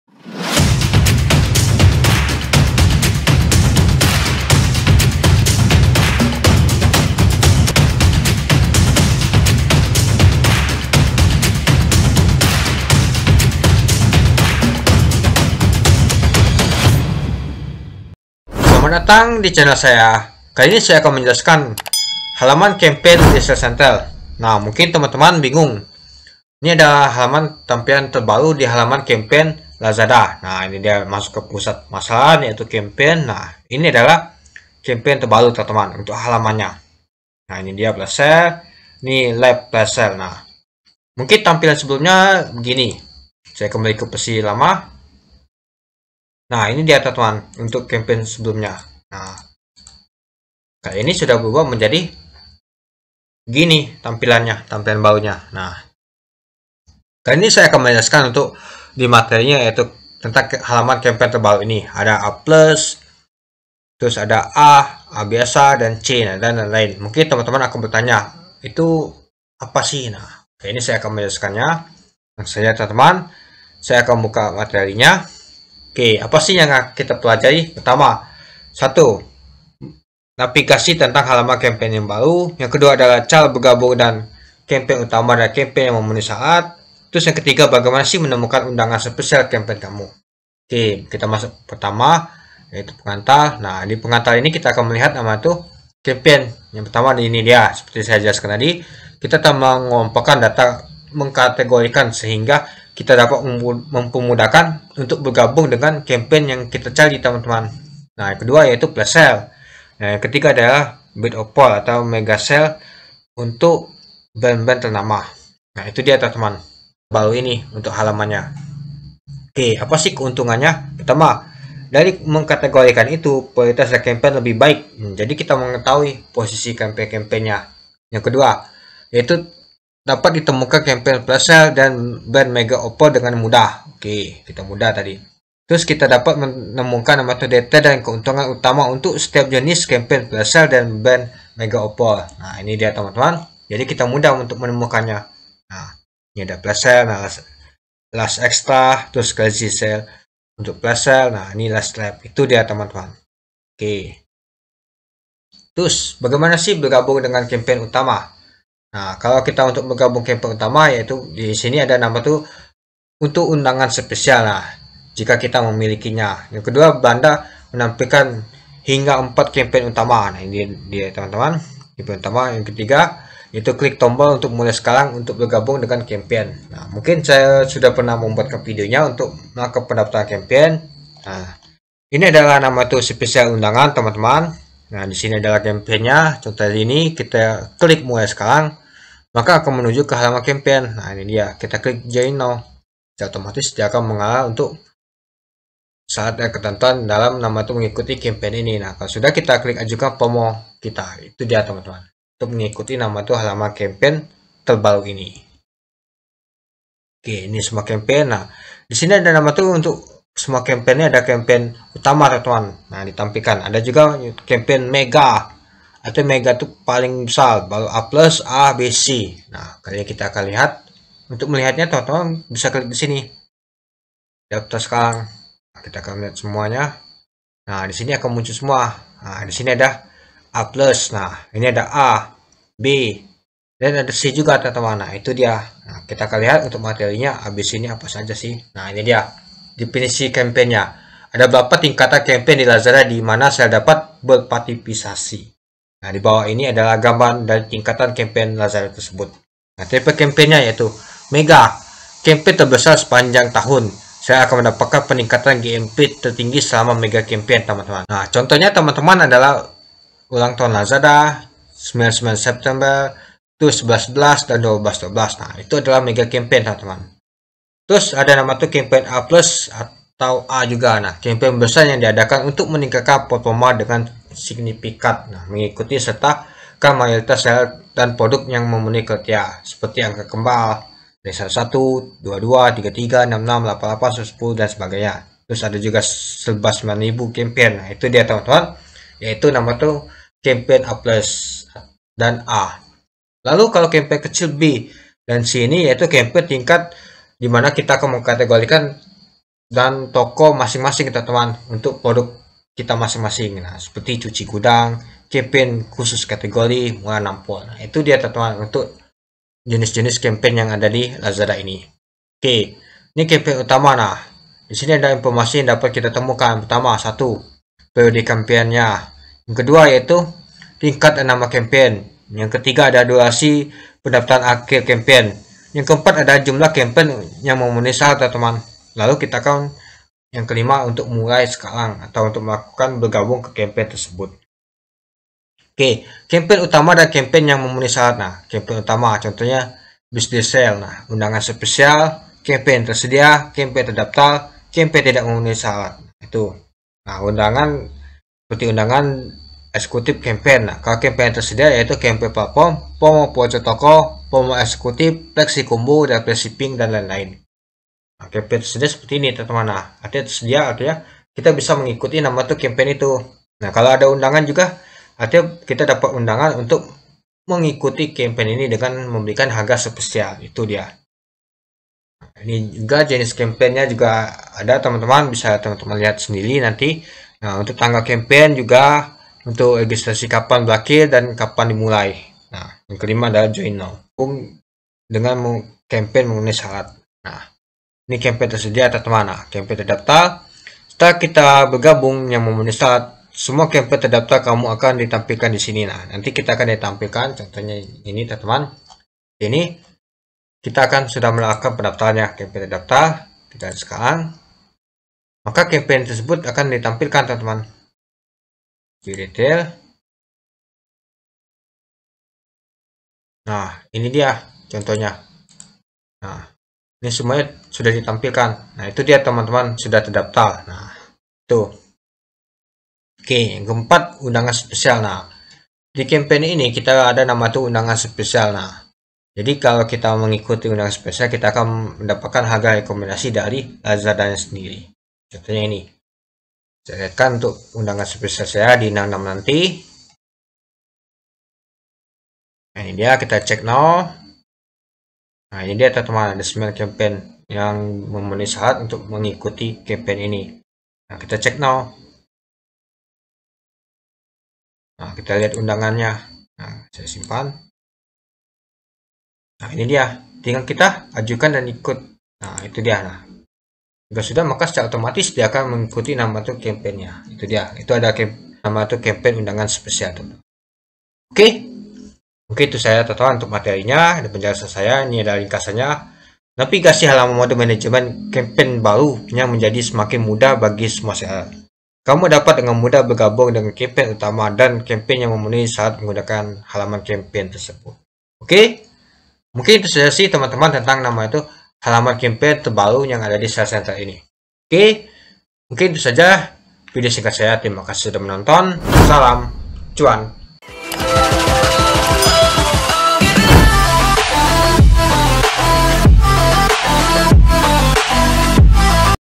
Halaman Campaign di Seller Center Lazada
Di video ini seperti biasa saya akan menjelaskan tentang: Halaman Campaign di Seller Center Lazada